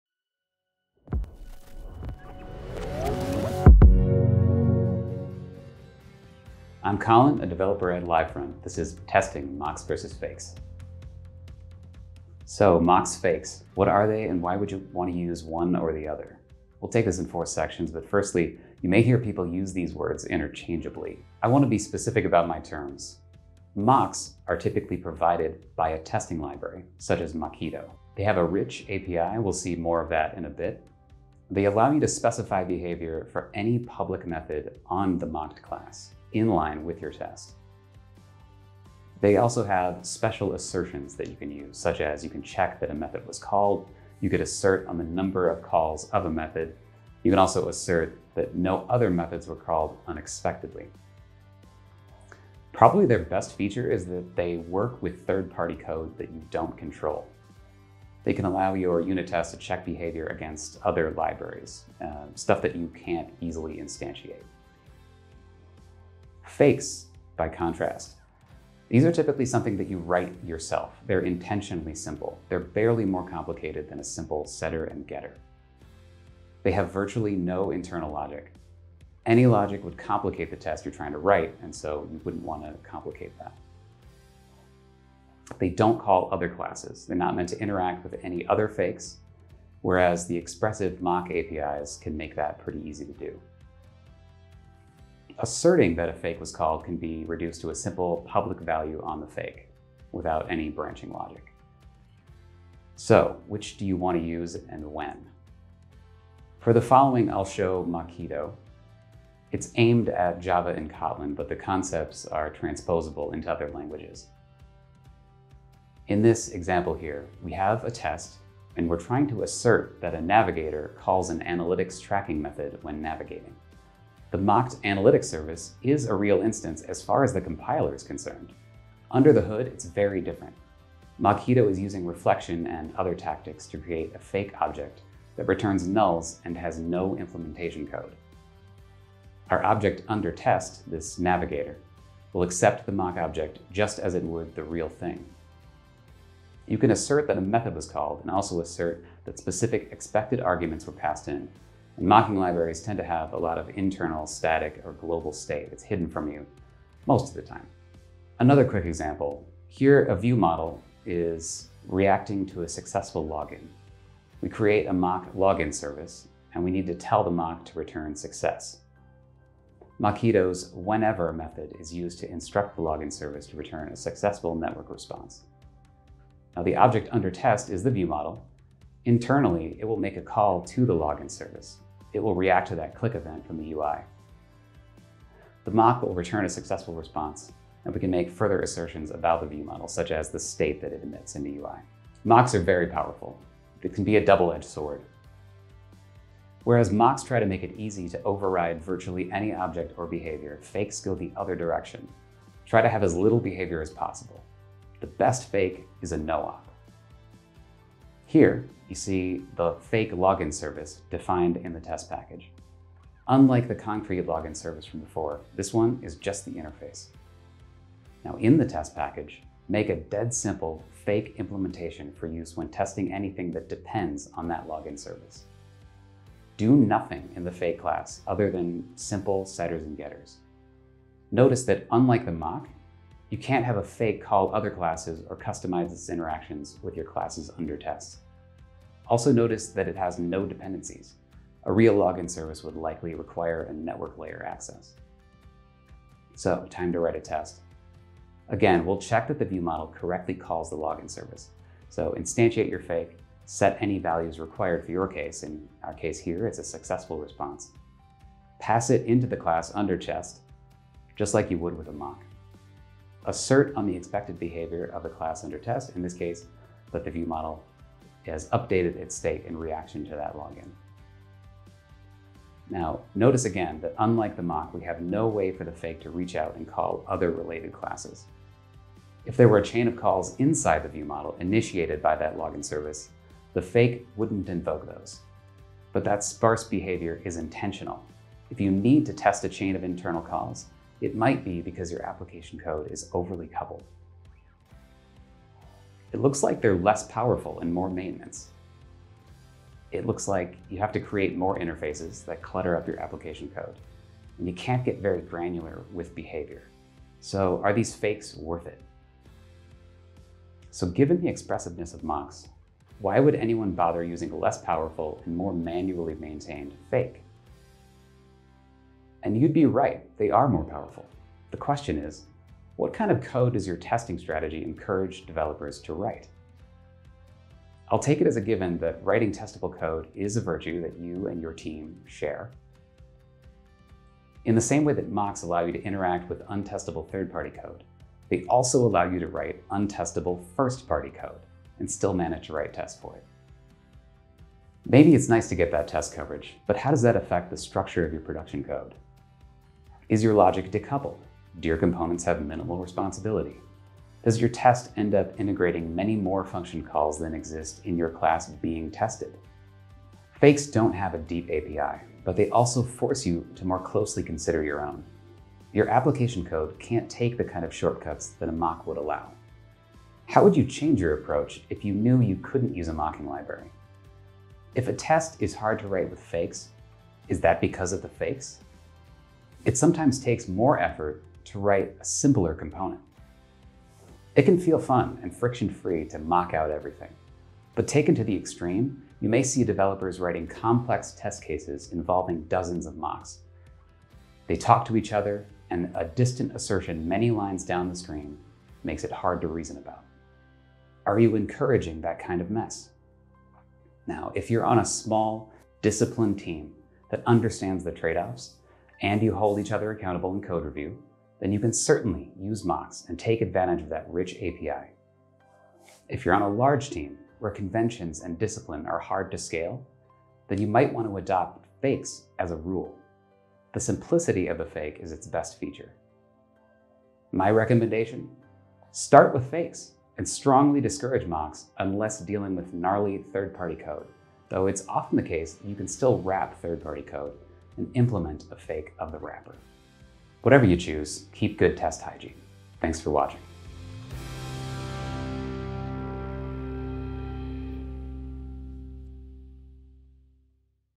Testing: Mocks vs Fakes | Livefront Talks 2023
Are you an engineer trying to decide between Mocks or Fakes for your test APIs? This video from the team at Livefront will provide an overview of Mocks and Fakes and various inputs to consider for each technique.

The best software products are tested and reliable. So whichever direction you choose, your end users will appreciate that you have a testing strategy!

If you have more questions along the way, you can find us at Livefront.com!